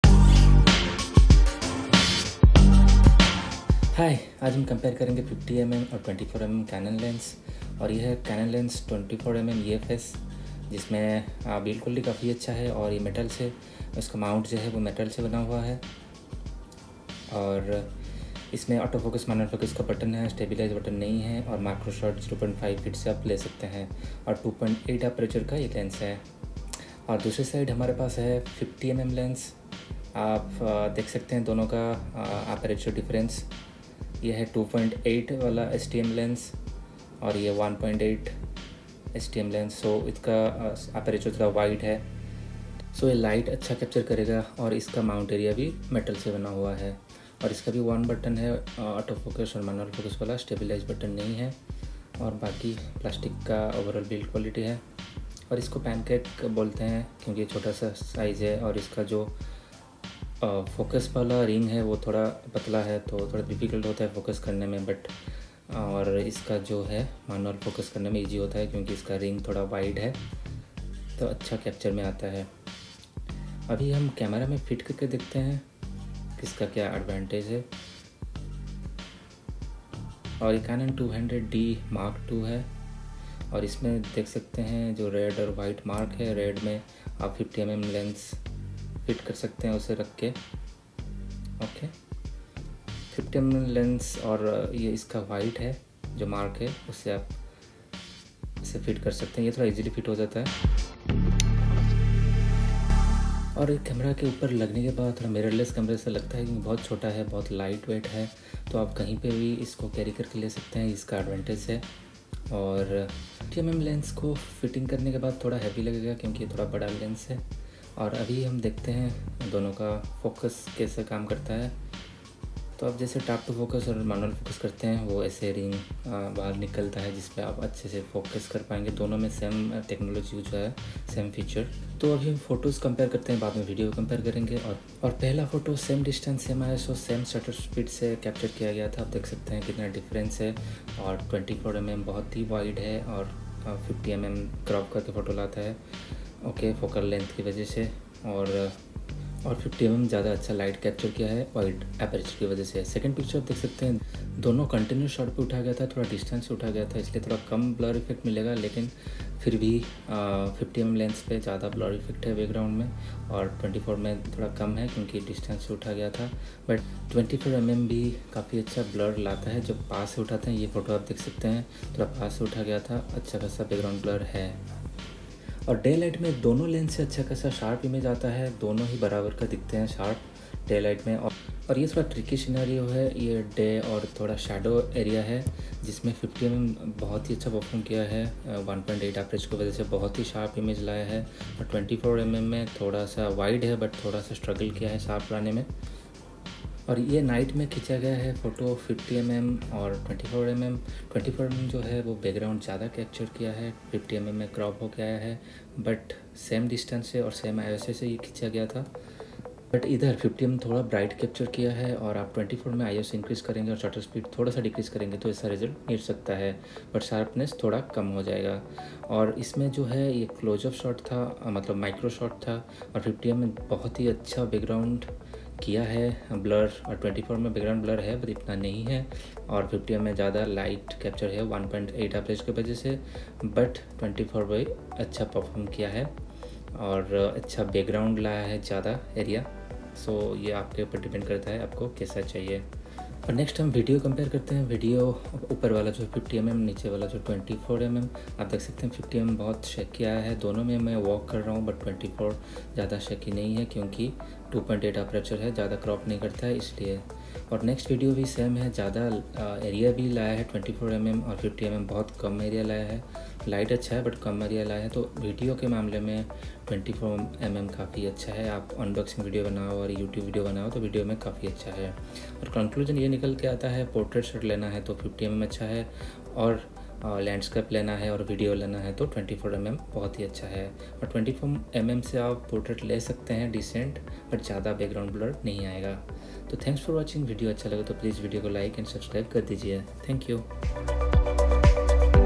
Canon 24mm 2.8 lens vs Canon 50mm 1.8 lens , Picture and Video comparison
In his video we will compare Canon 24mm vs Canon 50mm 1.8 lens , Picture and Video Test with Canon 200D mark 2. 24mm Portrait Shots

Canon EF-S 24mm f/2.8 STM Lens
Canon EF50MM F/1.8 STM Lens
Canon EF-S 55-250mm

Gadgets used in this video.(Buy Link)

Boya Mic
Camera Canon 200D Mark ii
Ring Light